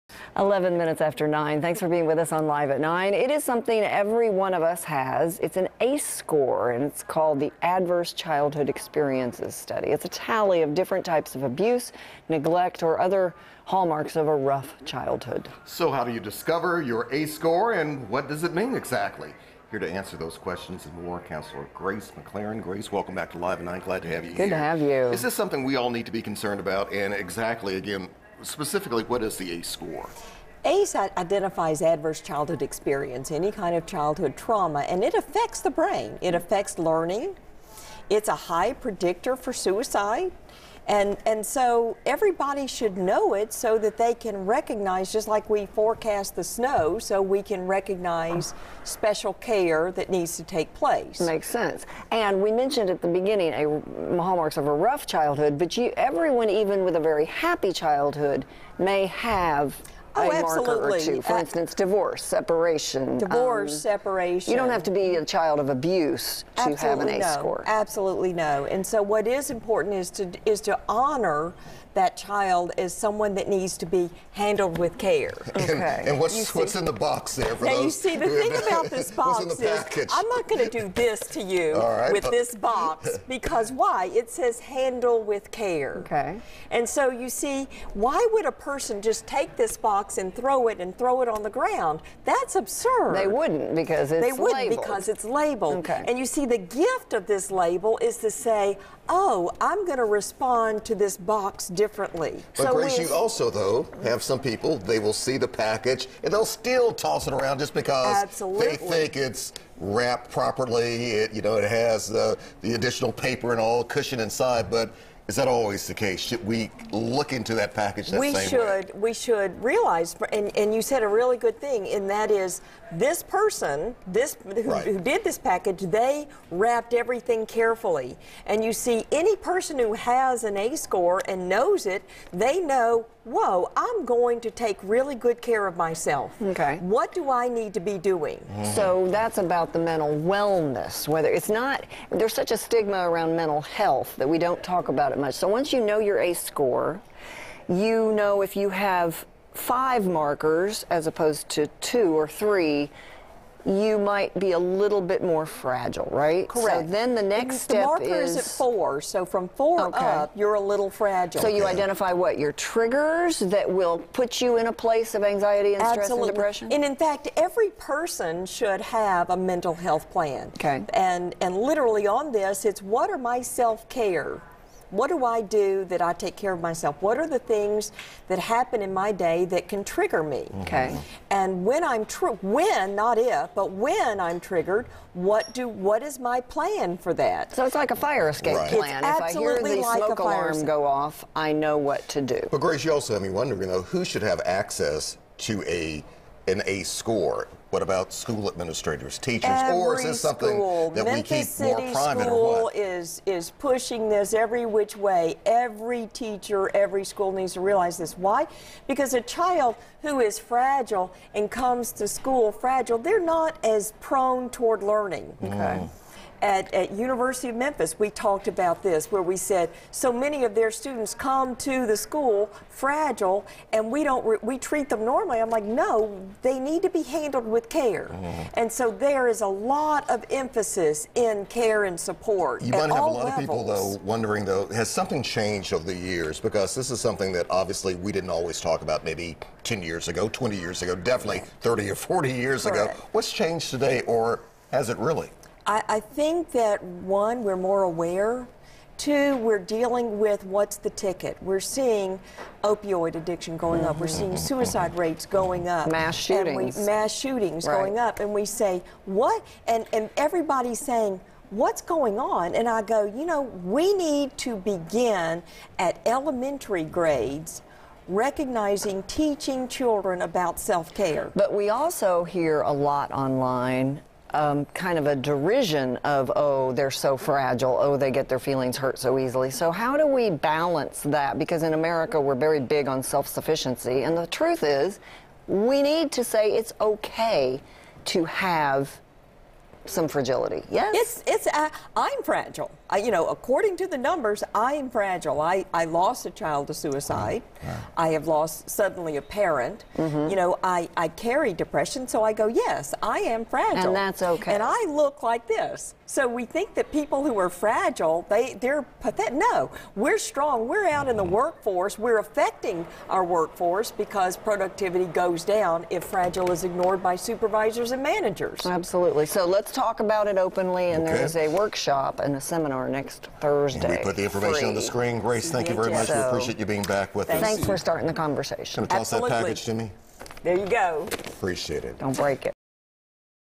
The ACE study
The Adverse Childhood Experiences study is a tally of different types of abuse, neglect and other hallmarks of a rough childhood.Counselor Grace McLaren tells us how to discover our score and what it means.